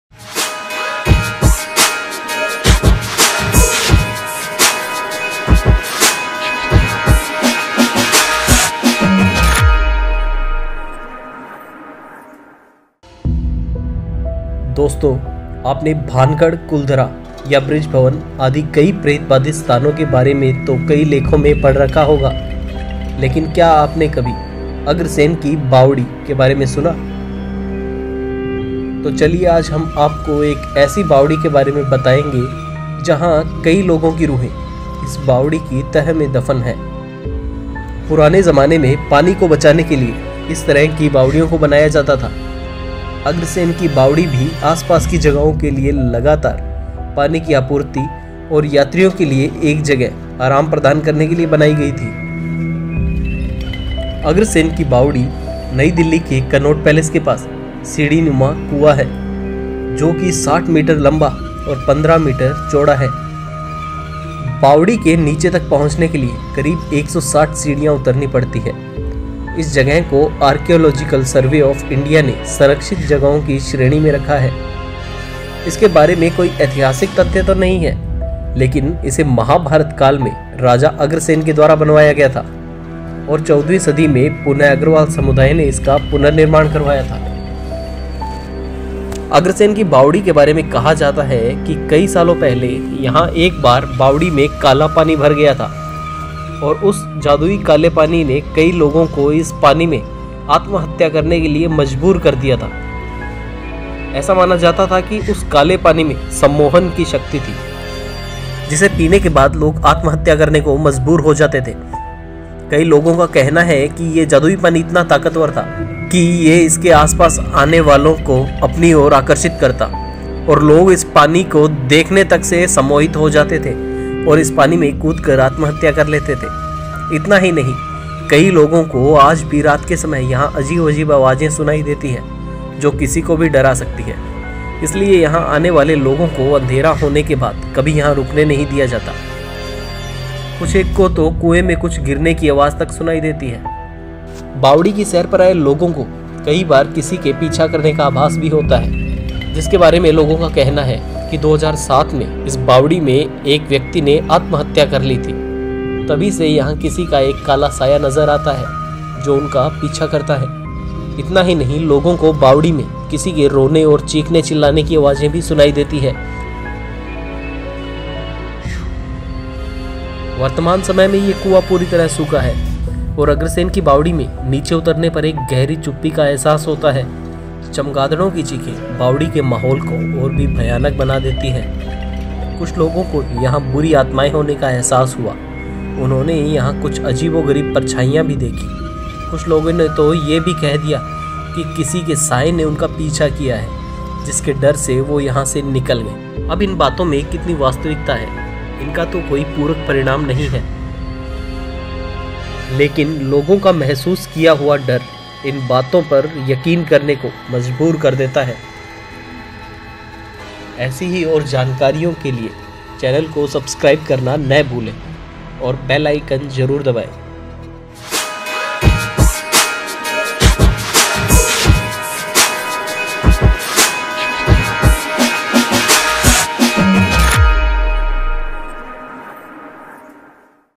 Agrasen Ki Baoli | Agrasen Ki Bawadi | Haunted Delhi
आपने भानगढ़ , कुलधरा या बृजराज भवन आदि कई प्रेतबाधित स्थानों के बारे में तो कई लेखो में पढ़ रखा होगा लेकिन क्या आपने अग्रसेन की बावरी Agrasen Ki Baoli के बारे में सुना है तो आज हम आपको एक ऐसी बावरी के बारे में बताएँगे जहा कई लोगो की रूहें इसके तह में दफन है.